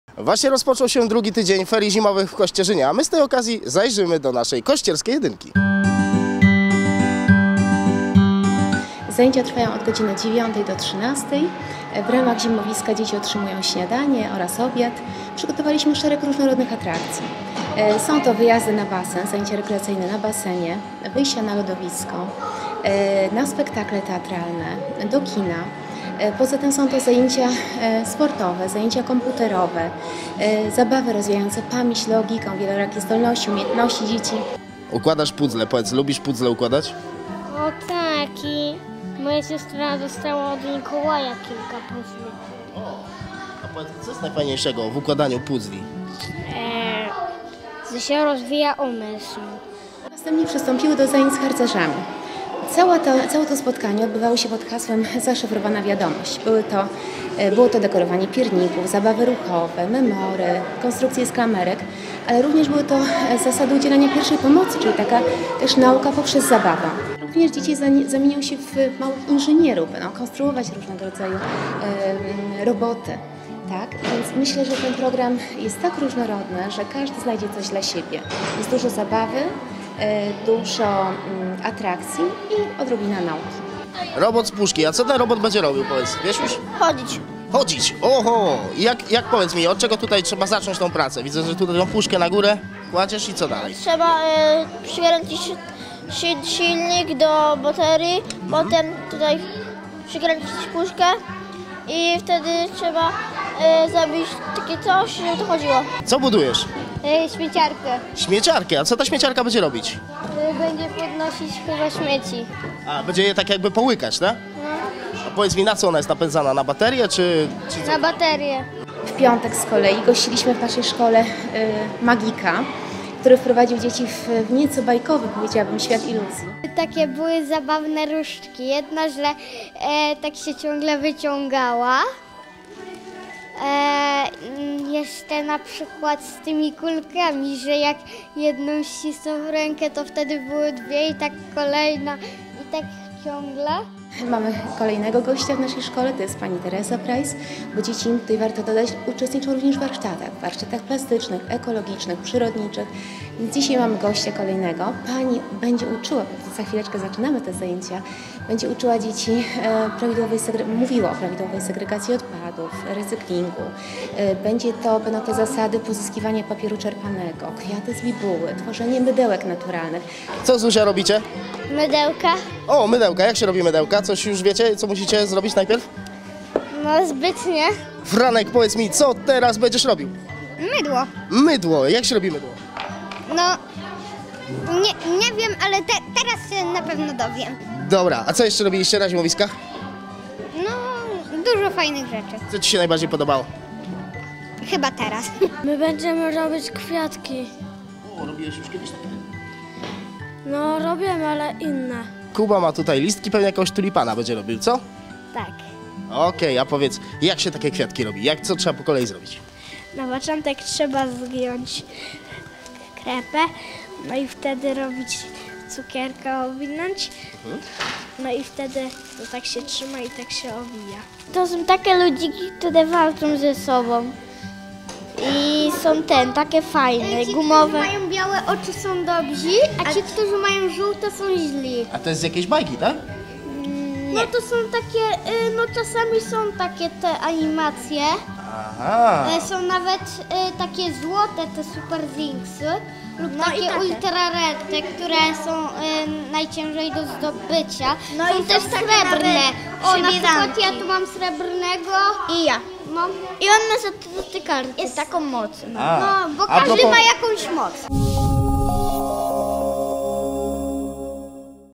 Zimowiska w kościerskiej "jedynce"
Rozpoczął się drugi tydzień ferii zimowych. Dzisiaj postanowiliśmy odwiedzić dzieci ze Szkoły Podstawowej nr 1 w Kościerzynie

Jeżeli uważasz, że to co robimy jest dla Ciebie istotne, możesz nas wspomóc